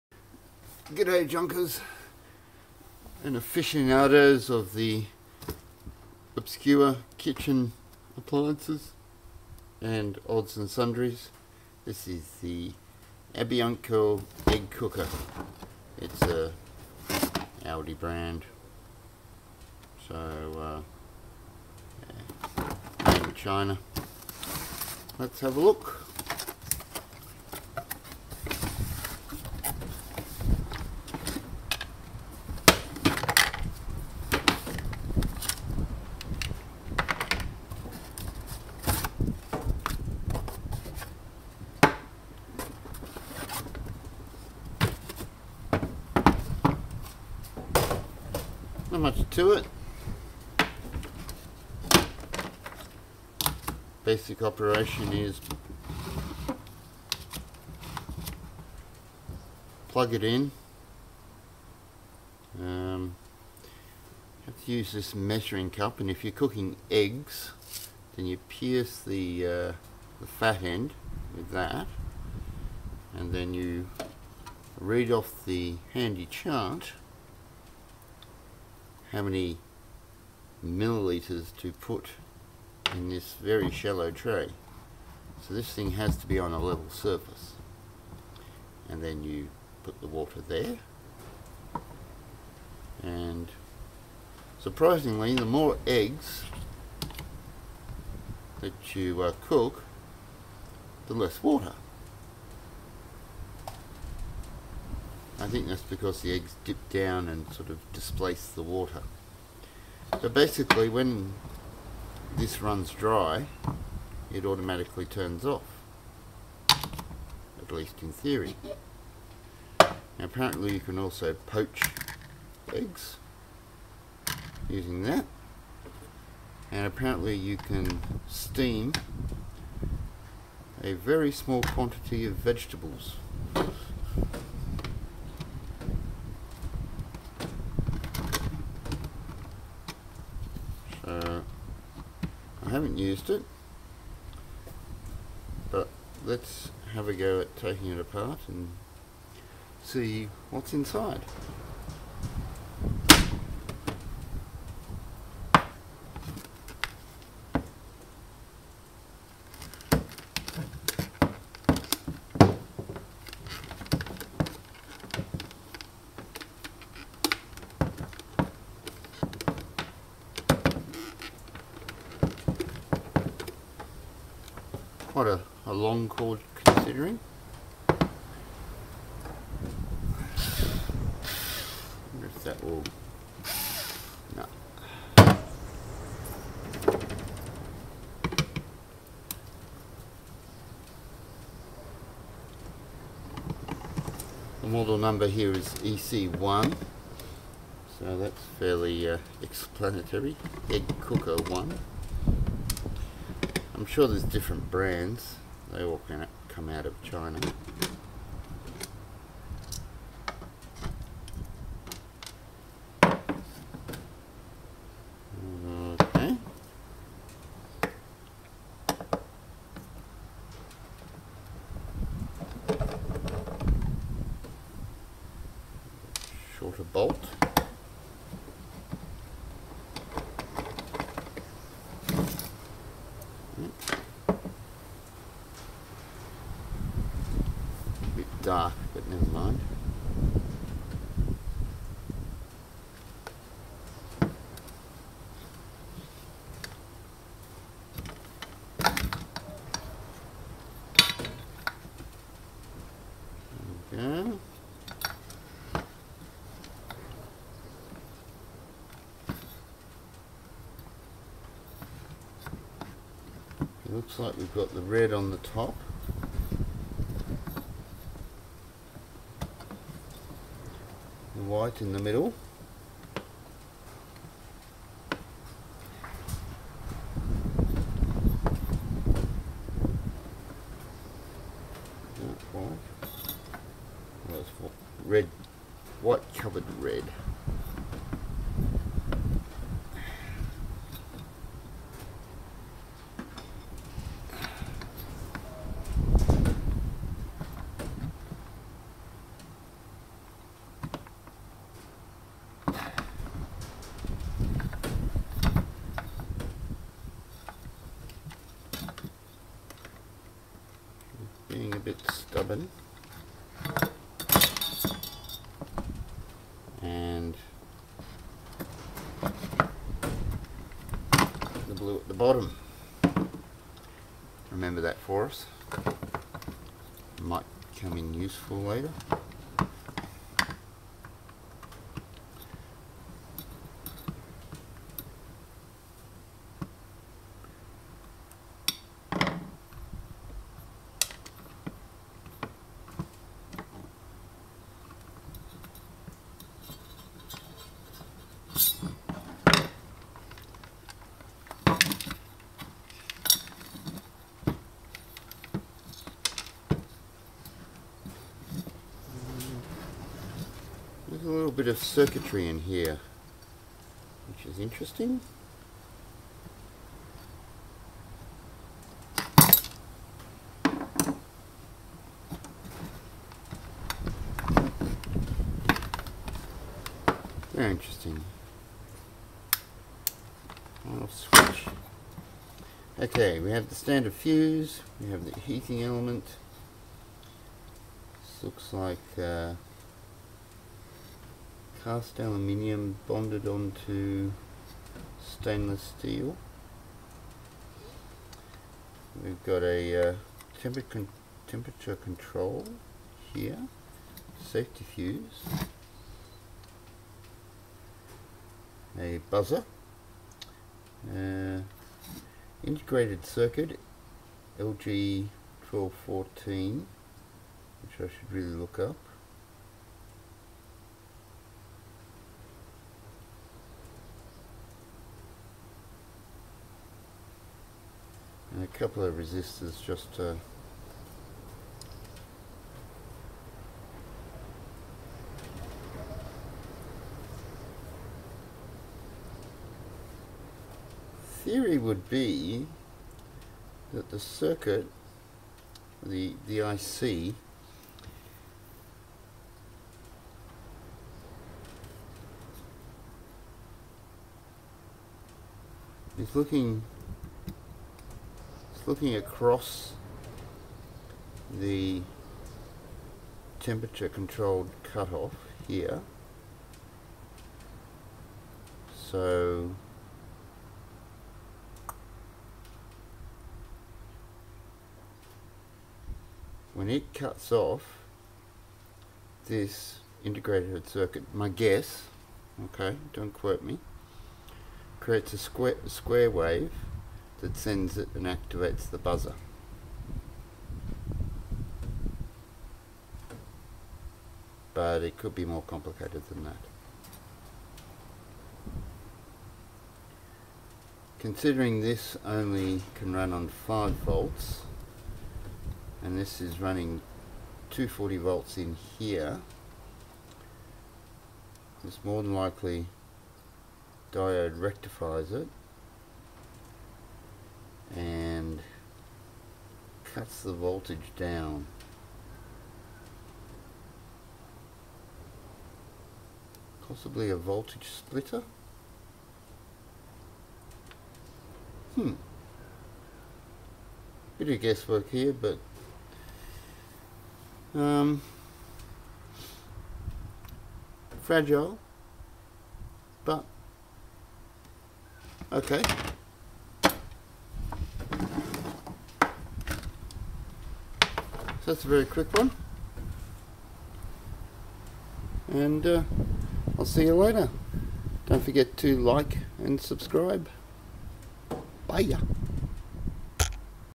Teardown Ambianco Egg Cooker EC1
I Teardown an Ambianco Egg Cooker EC1